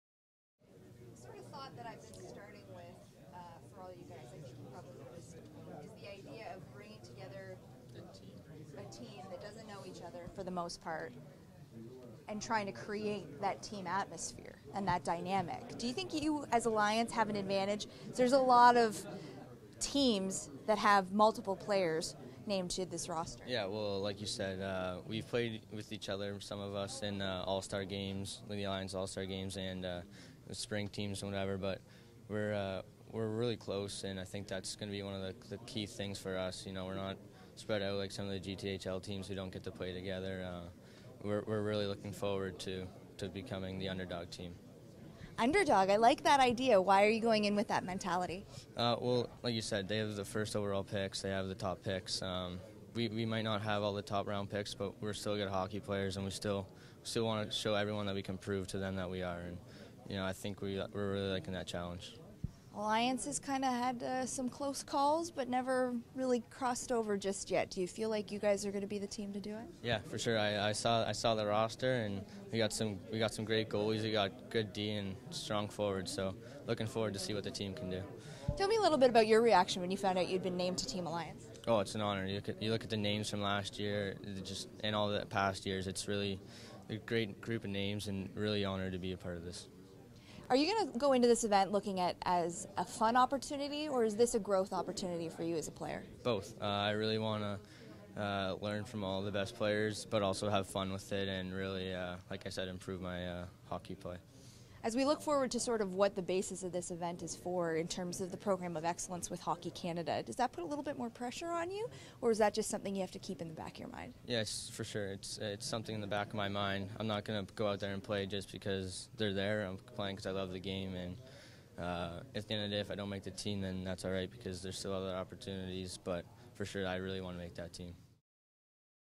OHL Gold Cup - Mitchell Hoelscher Interview
Mitchell Hoelscher is interviewed after being named to Team Alliance for the 2016 OHL Gold Cup. (Raw video)